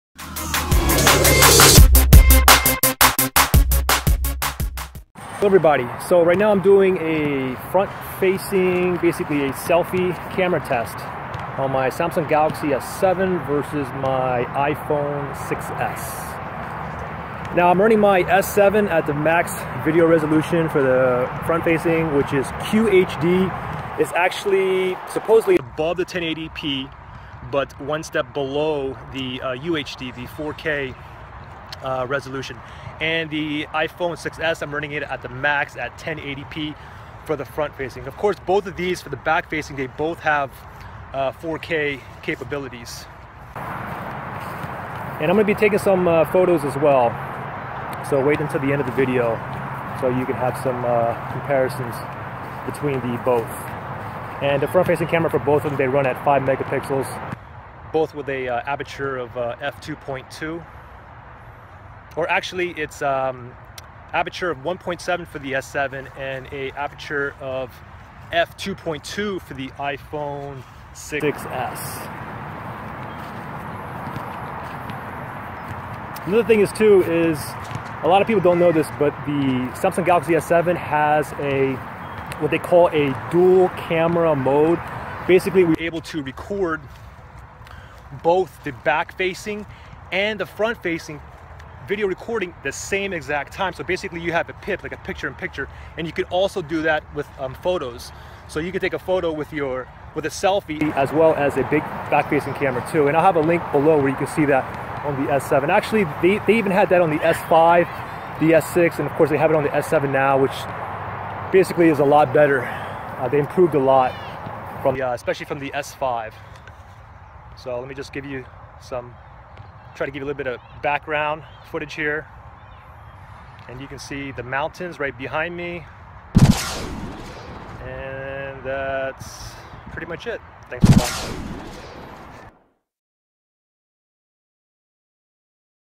Galaxy S7 vs. iPhone 6s Selfie Video & Photo Comparison
This is an informational video where the Samsung Galaxy S7 & iPhone 6s run side by side comparing the front facing camera (selfie) for video, photos, and mic/audio. The S7 is running at its max selfie video recording resolution of QHD and the iPhone at its max of 1080p HD. Note that both the S7 and iPhone 6s have 4k video abilities on the back facing camera which I compared side by side in another video

These two phones have one of the best phone cameras on the market. Which one do you think is better?

Galaxy S7_______________________iPhone 6s
Released: March 2016__________Released: Sept 2015
Selfie QHD Video Res___________Selfie 1080p HD Video Res
Front Camera 5MP______________Front Camera 5MP
f/1.7 aperture___________________f/2.2 aperture
Angle of view: 26mm____________Angle of view: 29mm

Galaxy S7
iPhone 6s